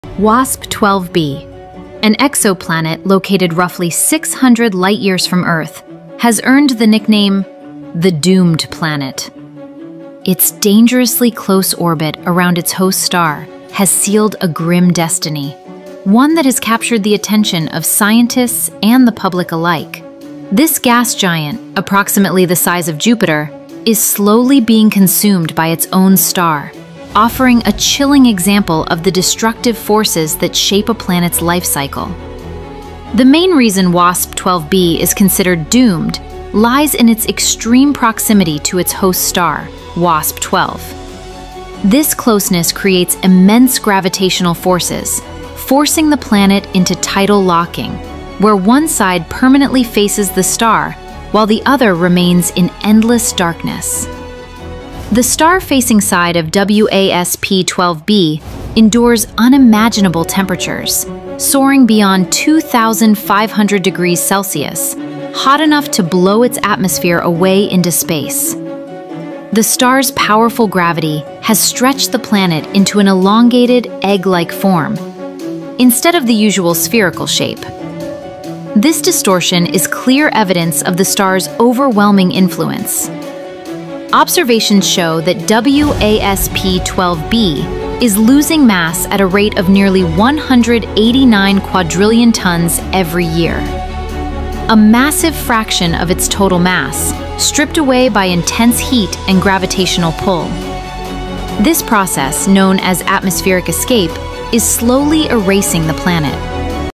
The most horrifying exo-planet💀
WASP-12b is not just another exoplanet.
It’s a world on the brink of destruction.

Located hundreds of light-years from Earth, this massive gas giant orbits so close to its star that its atmosphere is being torn away — stretched, heated, and slowly consumed.

Dayside temperatures soar beyond imagination.
Its skies glow with molten clouds.
And with every orbit, the planet loses more of itself to space.

Astronomers call it a doomed world — a glimpse into how extreme gravity, radiation, and time can erase an entire planet.

WASP-12b doesn’t just challenge our understanding of planets…
it warns us how violent the universe can truly be.